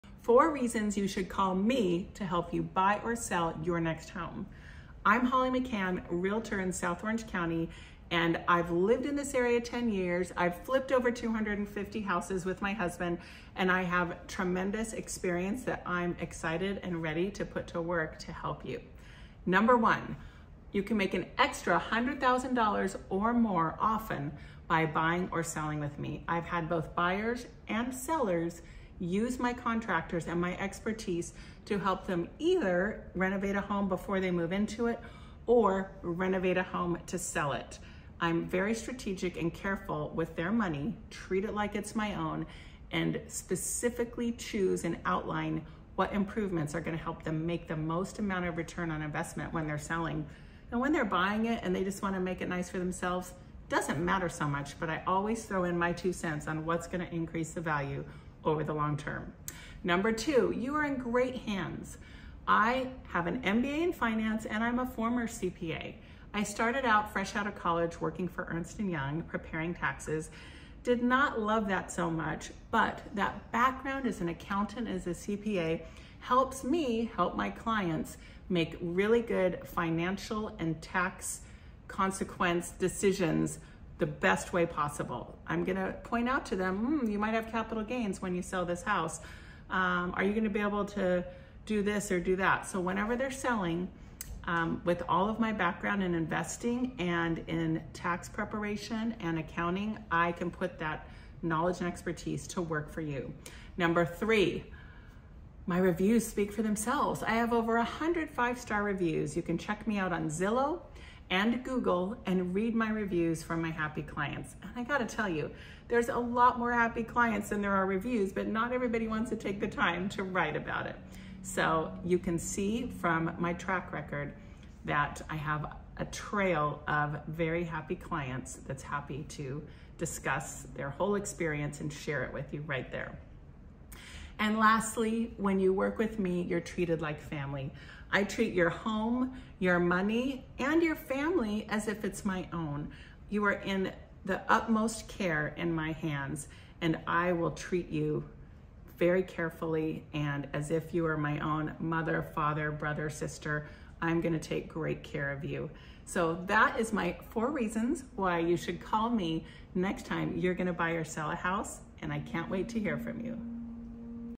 Top 1% Realtor in South Orange County, CA
Top 1% Realtor in South Orange County, CA - Holly McKhann

Four reasons you should call me to help you buy or sell your home in South Orange County, CA.

1. You can make an extra $100K or more by buying or selling a home with me because of my experience as a house flipper (I've flipped over 250 homes) and my connections to great contractors.

2. I have an MBA in finance and I am a former CPA. This finance experience helps me to help you make the best financial decisions possible.

3. I have over 100 five star reviews on Zillow and google. The reviews speak for themselves, people are very happy about their experience working with me.

4. You are treated like family when you work with me. I treat your money and your situation as if you were my brother or sister. I want only the best for you and will be fierce on your behalf.

I'm Holly McKhann, San Clemente realtor and house flipper.

OCHolly.com
McKhann Group Real Estate - San Clemente, CA

Holly McKhann - McKhann Group Real Estate
 🖥 OCHolly.com